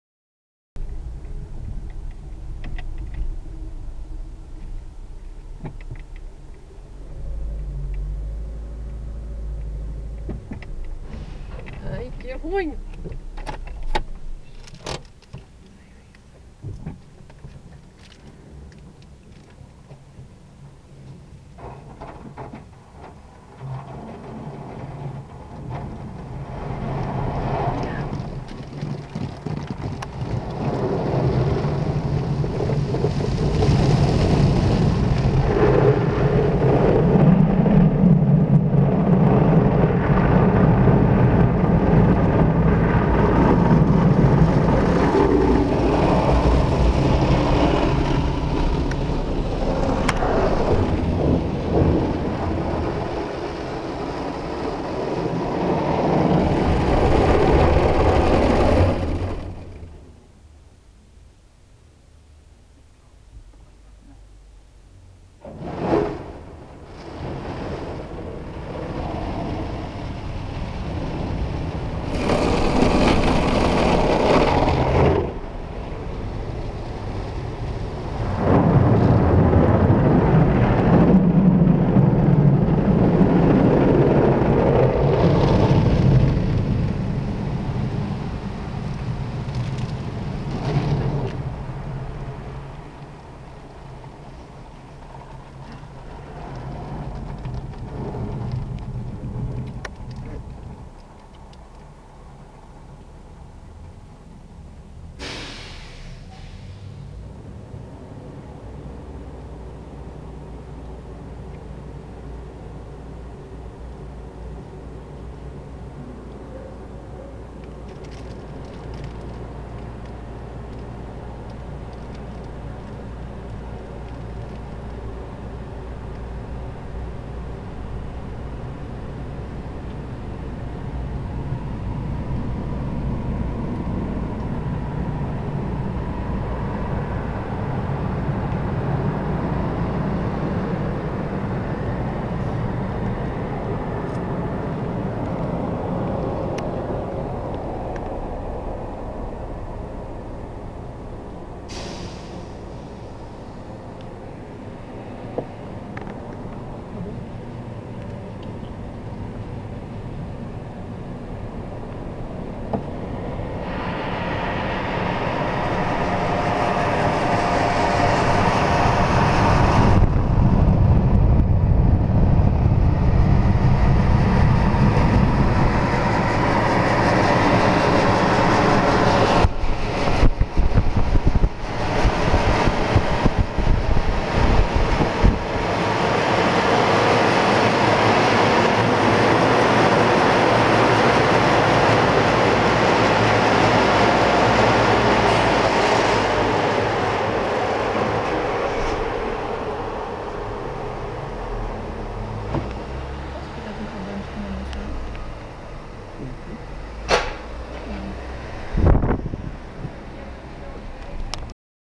Ceccato Ginga at Esso
Ceccato Ginga wd240 with red polyethylene  brushes at Esso petrol satation